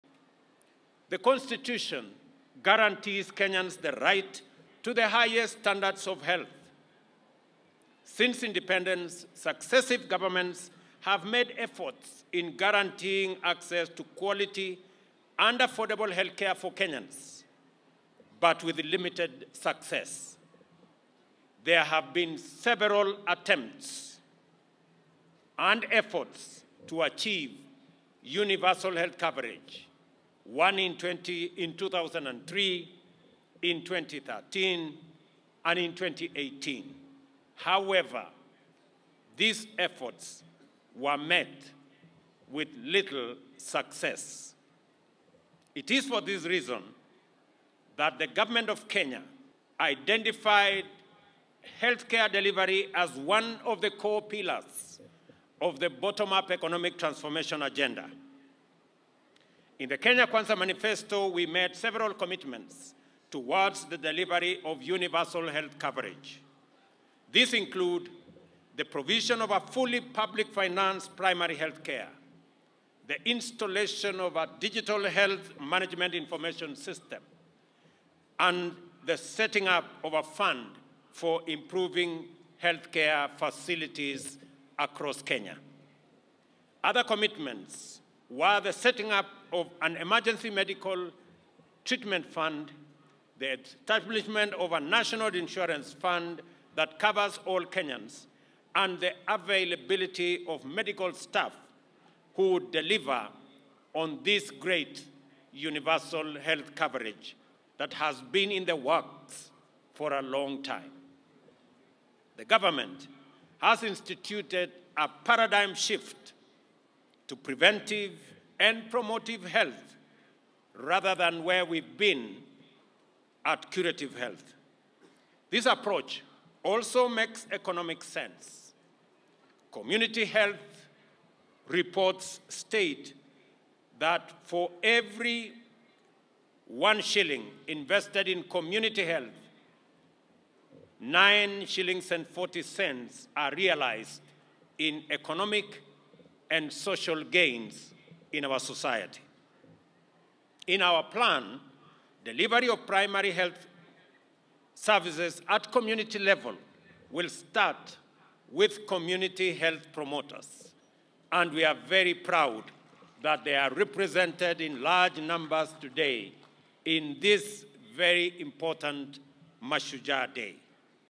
We will not fail on Universal Health Coverage this time round - President Ruto
President William Ruto during the celebrations to mark the 60th Mashujaa Day at the Kericho Green Stadium, on 20th October 2023.

President Ruto said the Government is rebuilding its healthcare system, making it more efficient and effective. He said the new system will be less burdening and people-centred, noting that it will be progressive, responsive and sustainable.

The President pointed out that the medical care system will be supported by a strong team of motivated, amply-equipped and adequately-trained health workers. He maintained that it is the Government’s plan that every Kenyan has access to quality care when they need it, without a strain on their income.